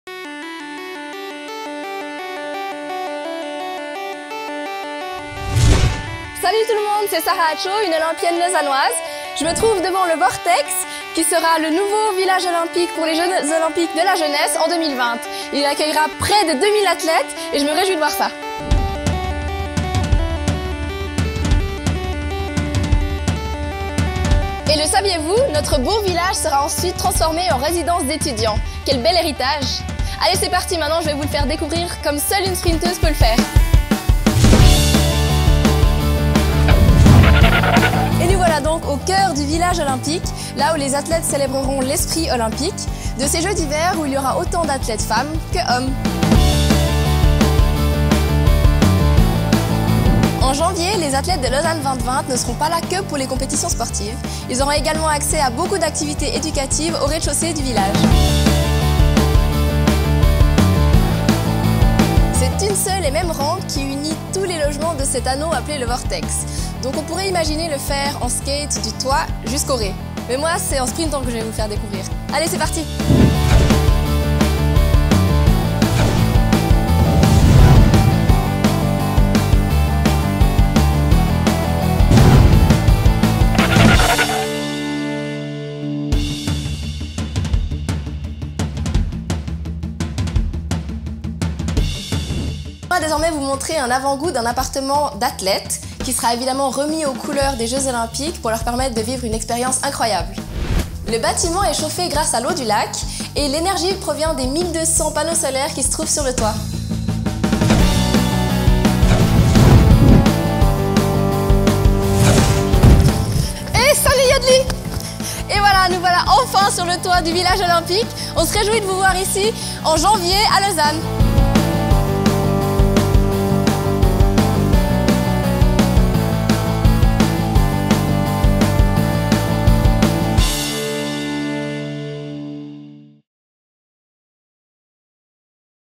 Sarah Atcho runs through the Lausanne 2020 Olympic Village
- Follow one of the fastest Swiss athletes, Sarah Atcho, in the future Olympic Village of the Lausanne 2020 Youth Olympic Games.